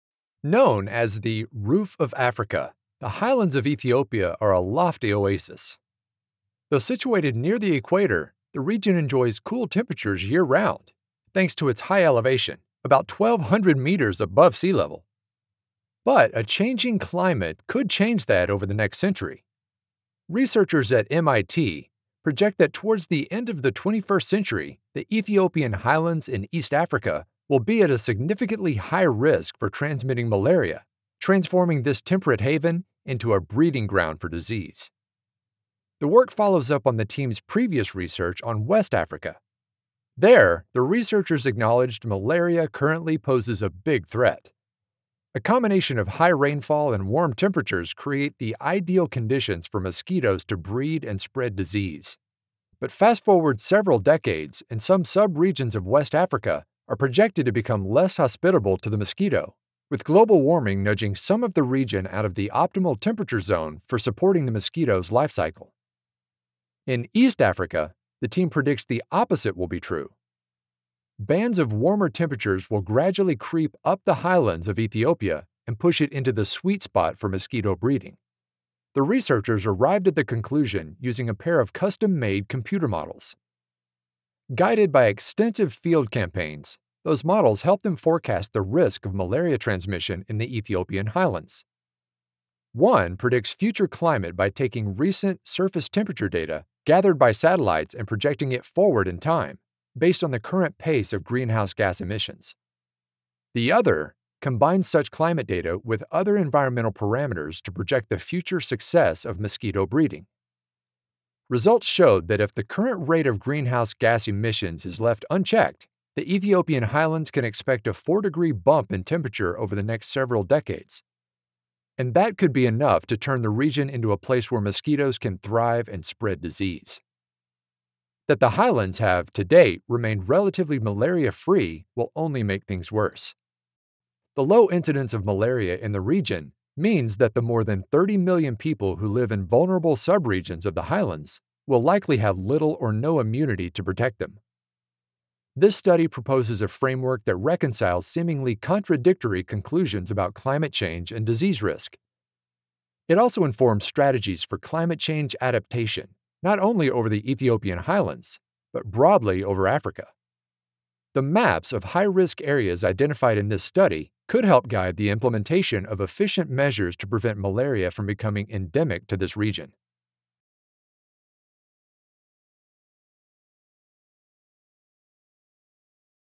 Climate Change impacts on Malaria in Ethiopia
Study: "Increased risk of malaria transmission with warming temperature in the Ethiopian Highlands".